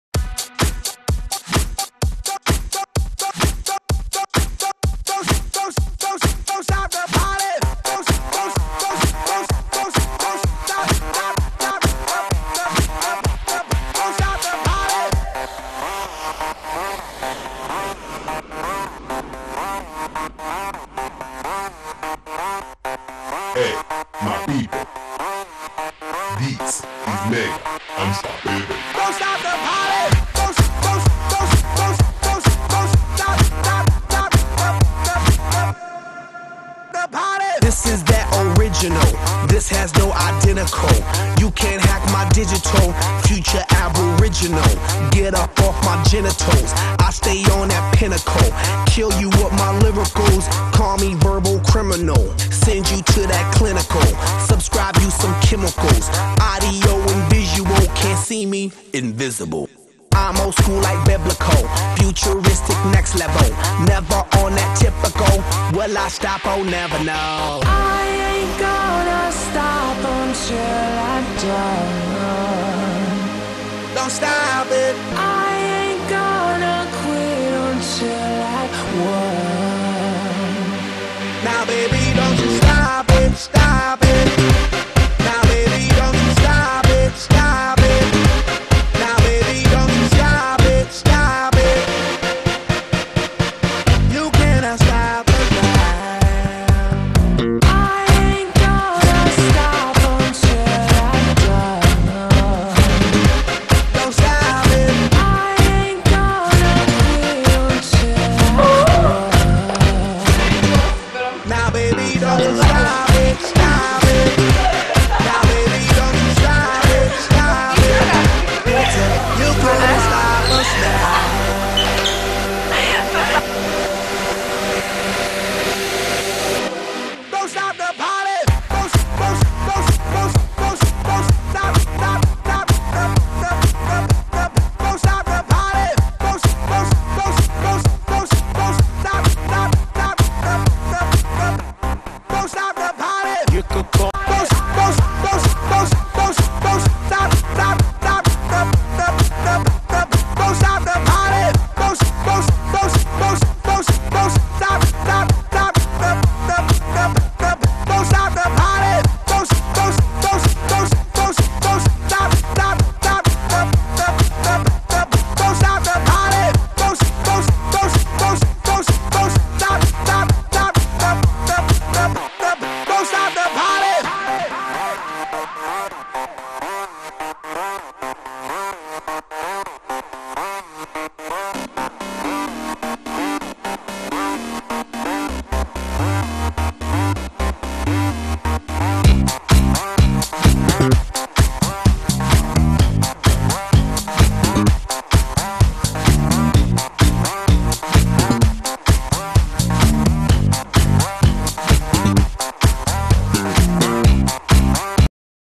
Dizzy Friday team makes a party in Lithuania
Dizzy Friday team makes a party in Lithuania after the Crew challenge competition. Enjoying Klaipeda :))) dance, fun, happiness :))